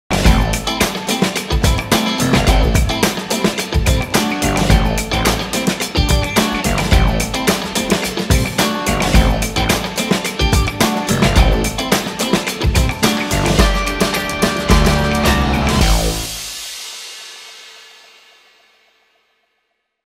Nowa Myśliwska. Nowe mieszkania Kraków Płaszów. 2 i 3 pokoje!
W 7-piętrowym budynku w kształcie litery L powstanie 129 mieszkań. Na elewacjach zobaczymy odcienie bieli, szarości i antracytu.